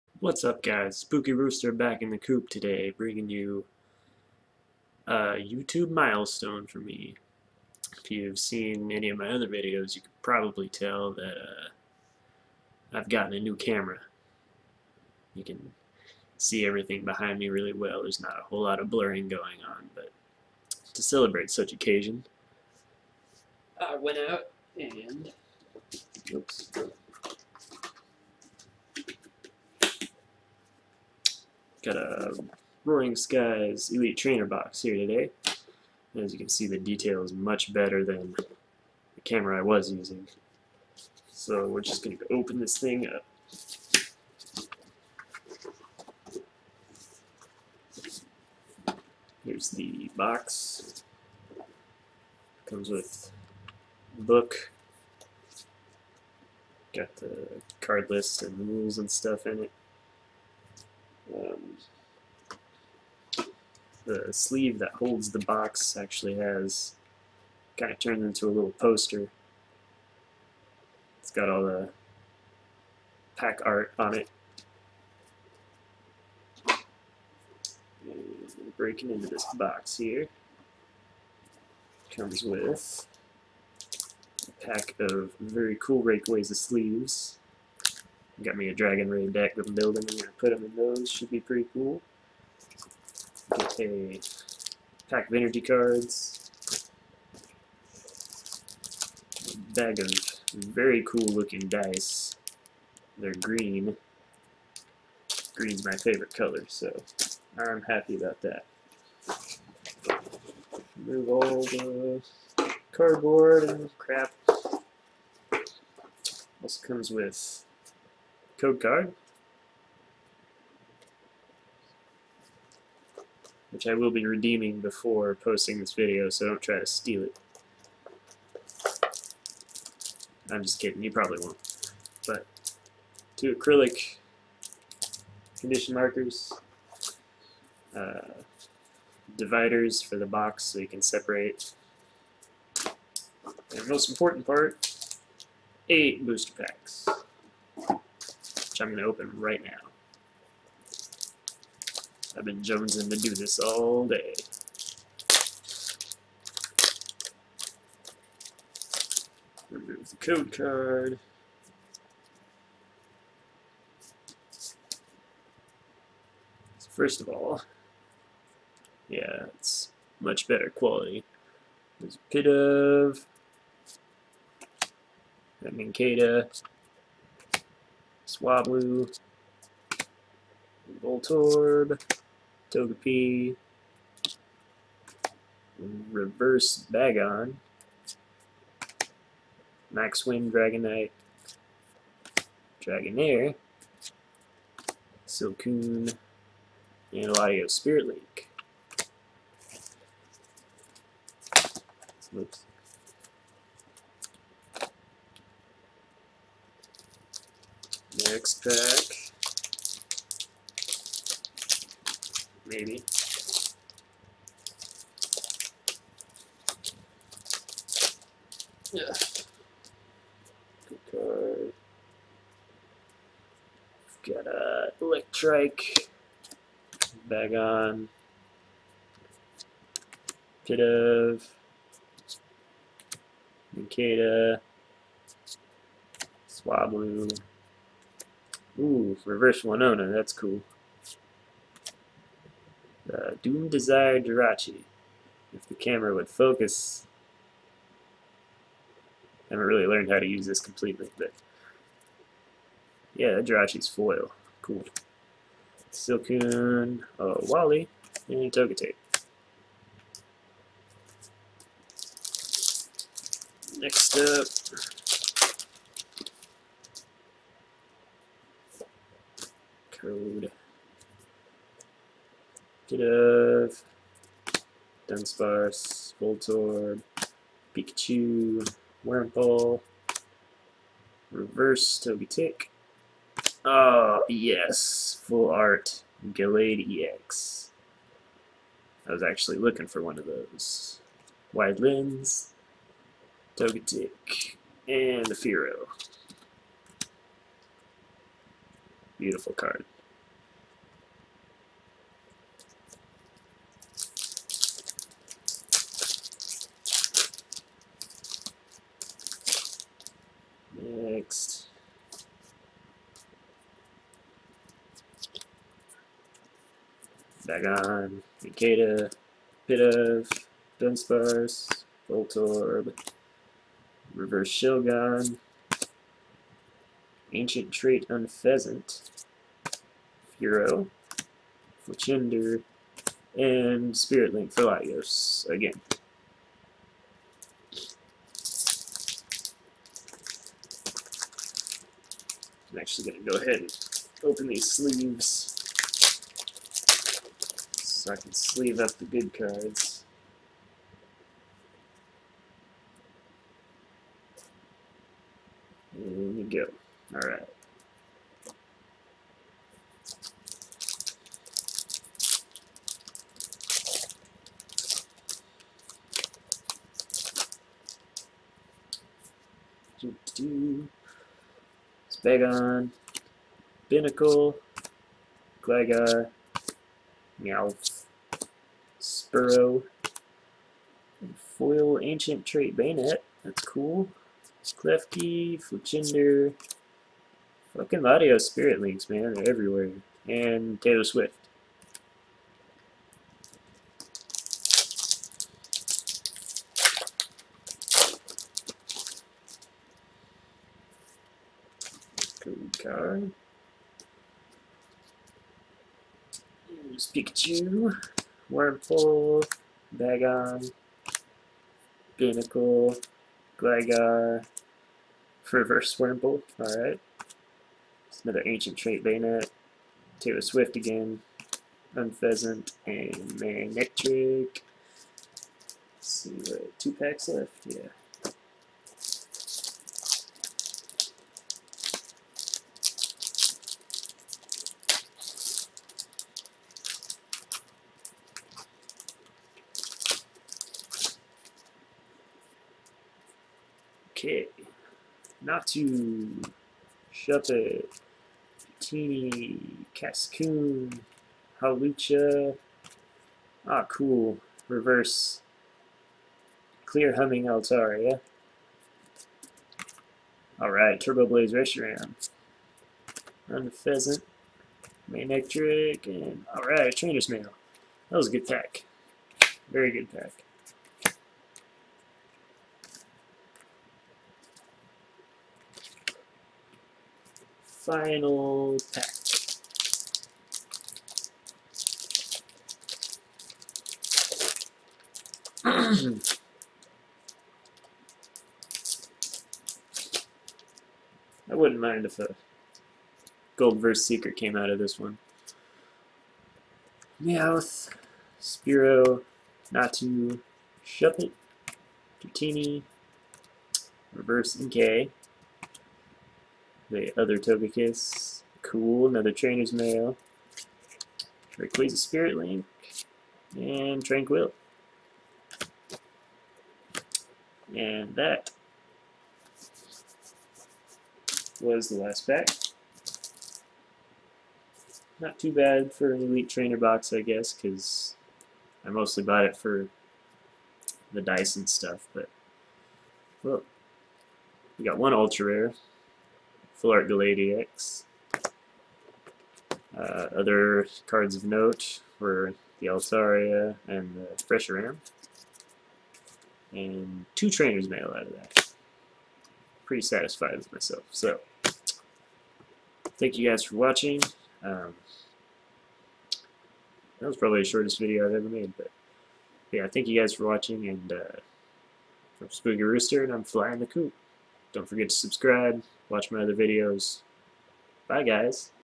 ROARING SKIES ELITE TRAINER BOX OPENING
Testing out my new cam while opening an Elite Trainer Box from Roaring Skies.